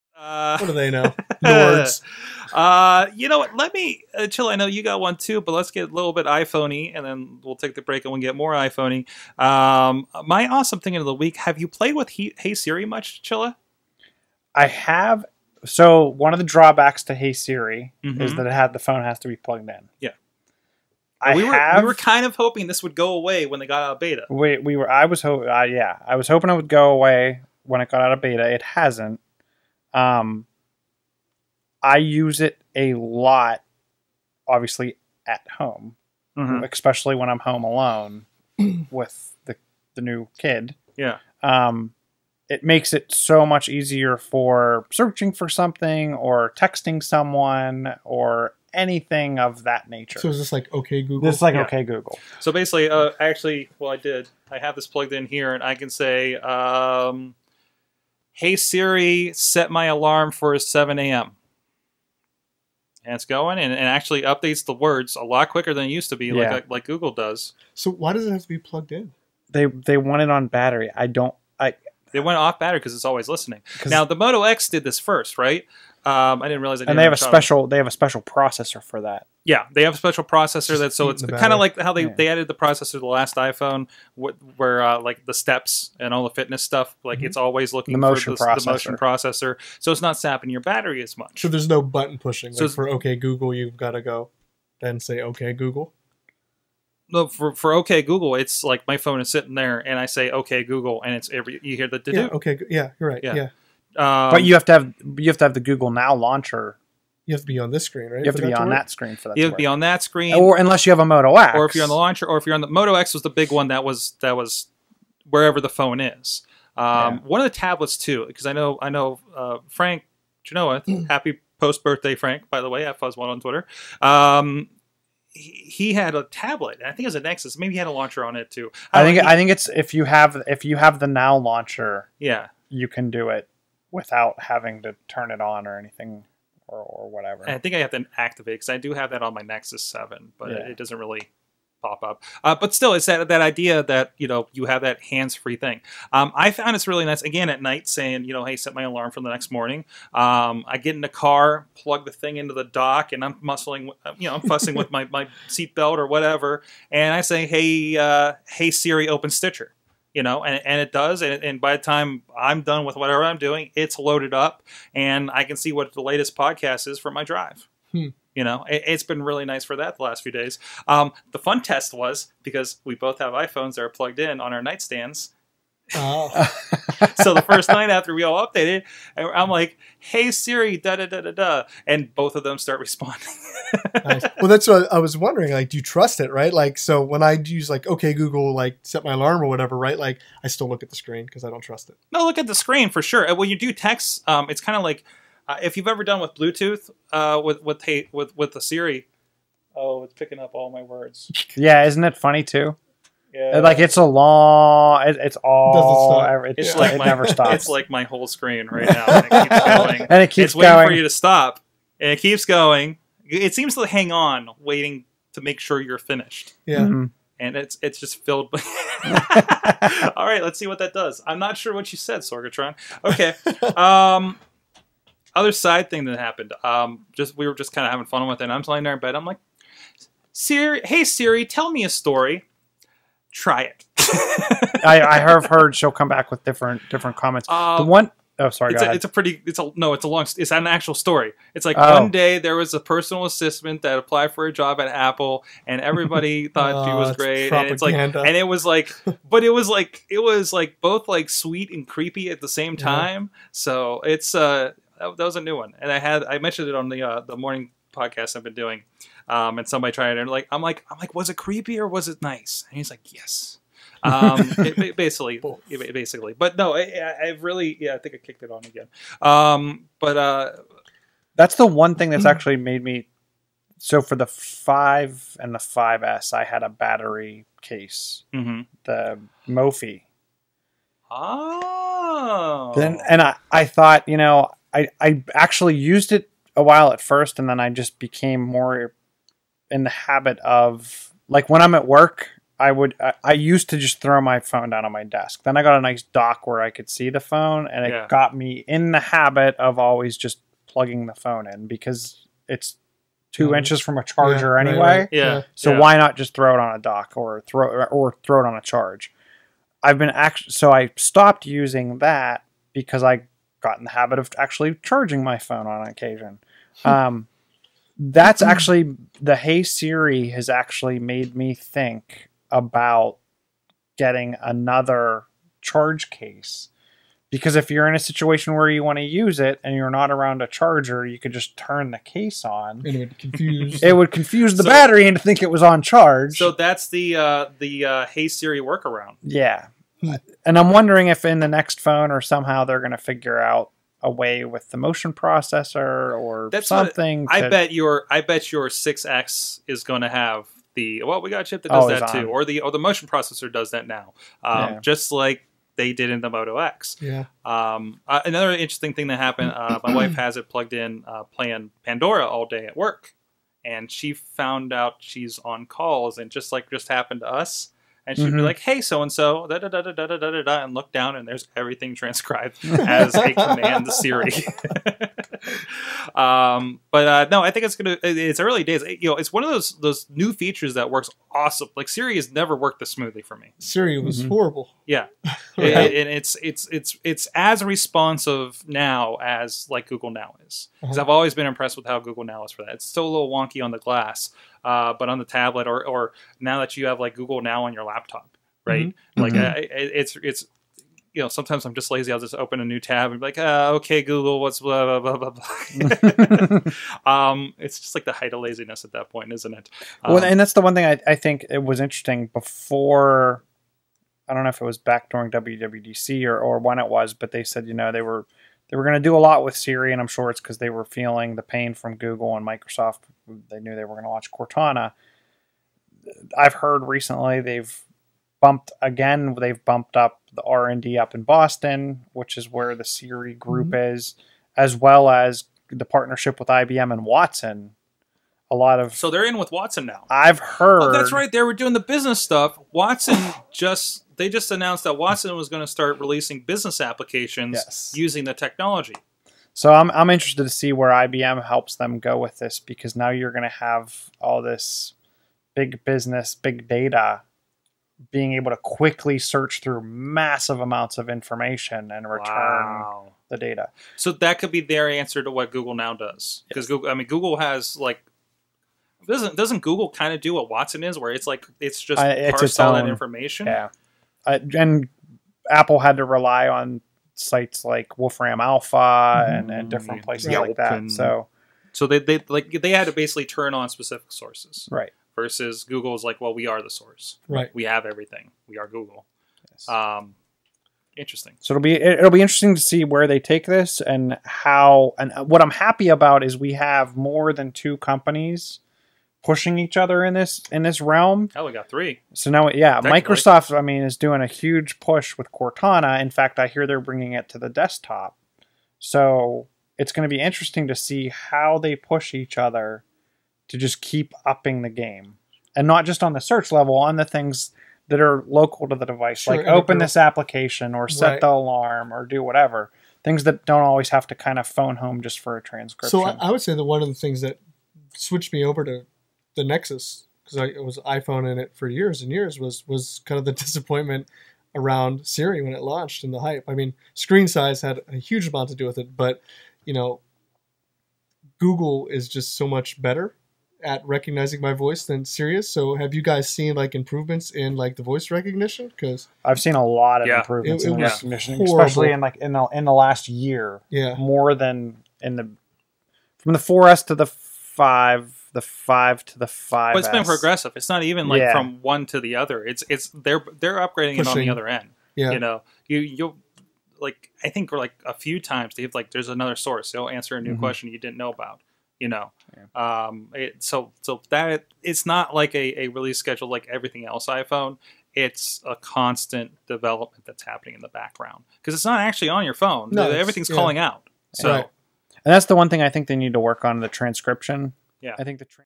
How Well Does "Hey Siri" work?
We talk about our experiences so far.

Follow these awesome people on Twitter: John Chichilla (@chilla), Josh Lucas (@workhardpgh), and Mike Sorg (@sorgatron).

Also, check out sorgatronmedia.com and awesomecast.com for more entertainment; and view us livestreaming Tuesdays at 6:30 PM EST!